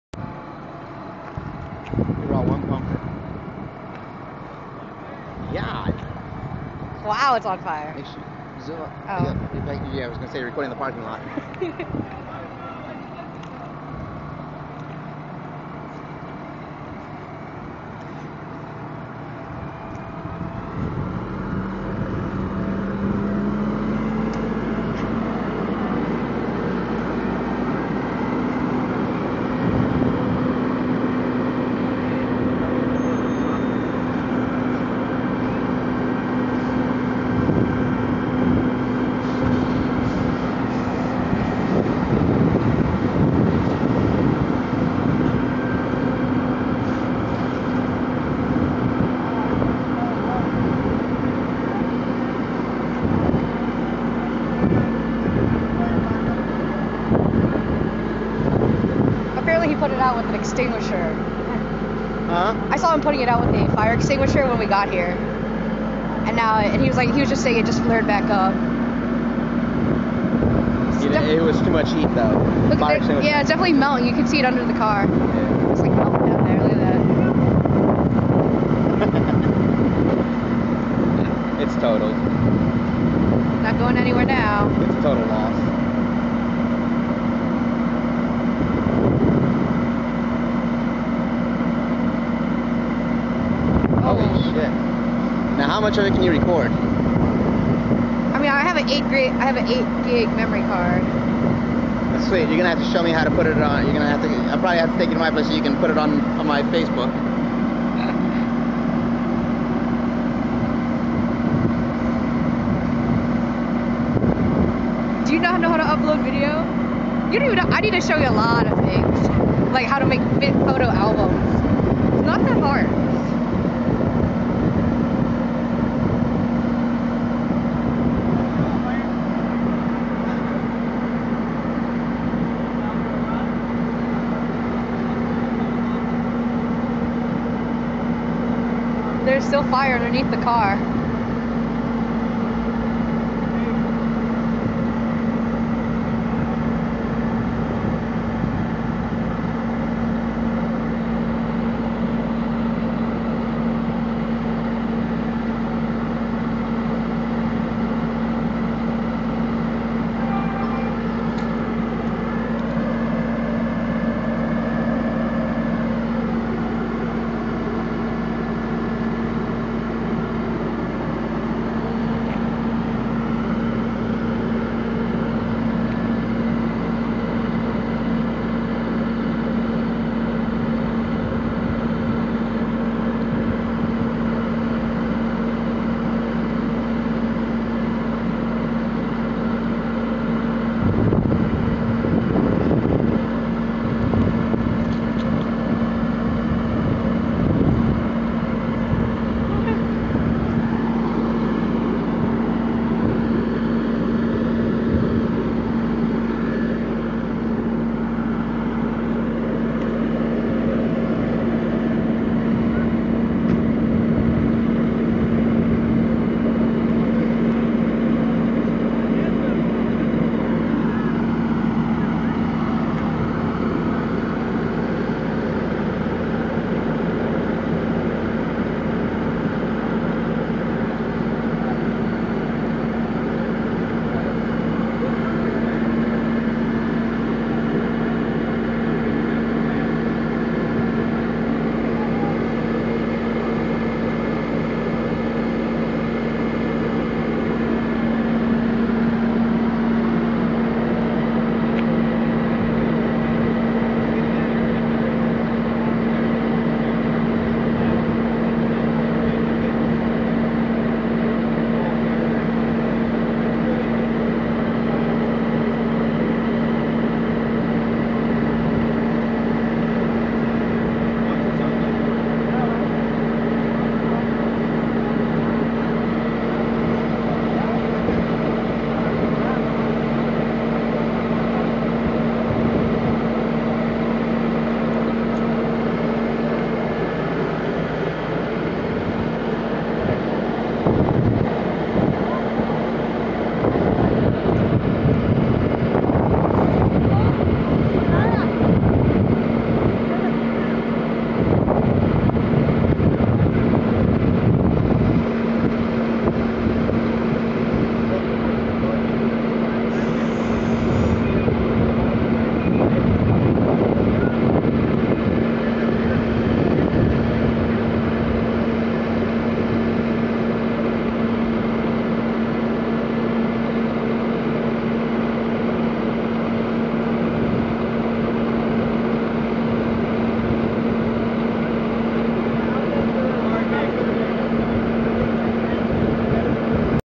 Car fire on rt 11 pt. 1
HFD arrives and conquers